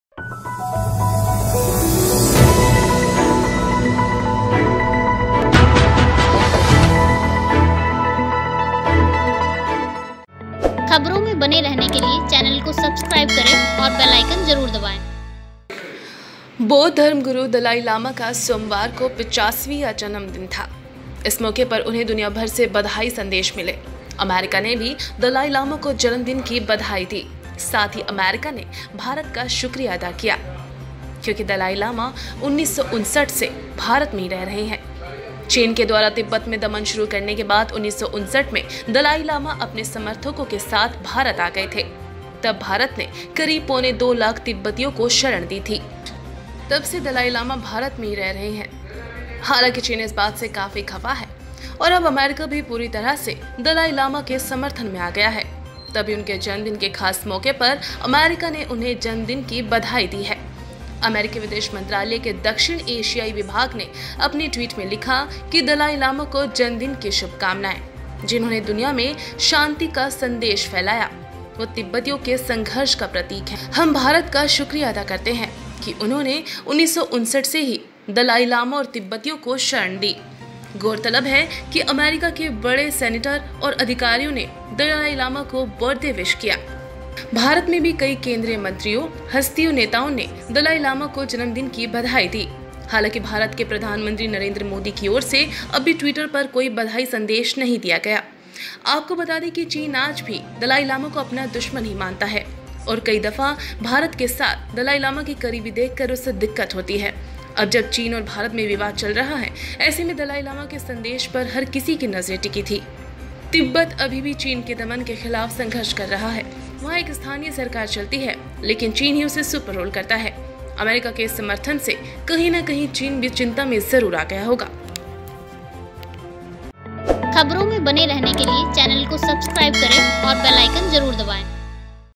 USA ne di dalailama ko badhayi दलाई लामा के समर्थन में अमेरिका, भारत ने भी कहा शुक्रिया
बौद्ध धर्मगुरु दलाई लामा का सोमवार को 85वां जन्मदिन था. इस मौके पर उन्हें दुनियाभर से बधाई संदेश मिले. अमेरिका ने भी दलाई लामा को जन्मदिन की बधाई दी, साथ ही अमेरिका ने भारत का शुक्रिया अदा भी किया. क्योंकि दलाई लामा 1959 से भारत में ही रह रहे हैं.